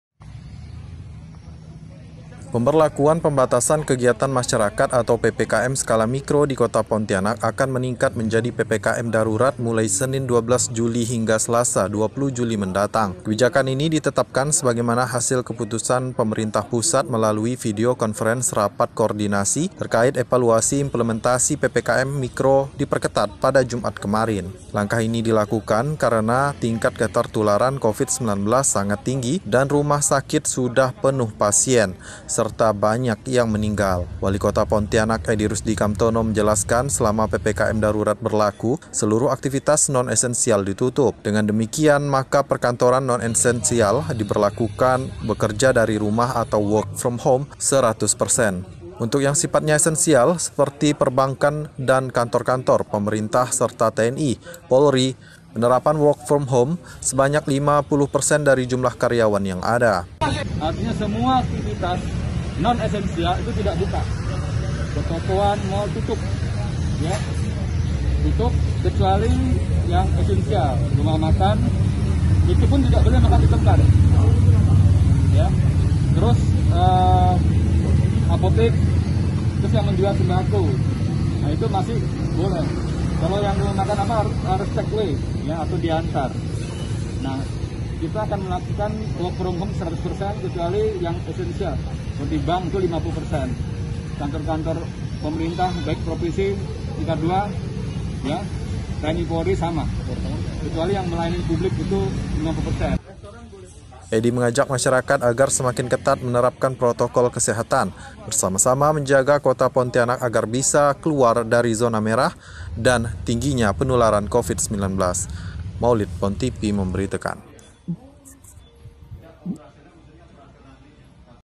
TERAPKAN PPKM DARURAT MULAI 12 HINGGA 20 JULI 2021
Pemberlakuan pembatasan kegiatan masyarakat atau PPKM darurat bakal diterapkan di Kota Pontianak, terhitung mulai Senin 12 Juli hingga Selasa 20 Juli mendatang. Selama PPKM darurat berlaku, seluruh aktivitas non esensial ditiadakan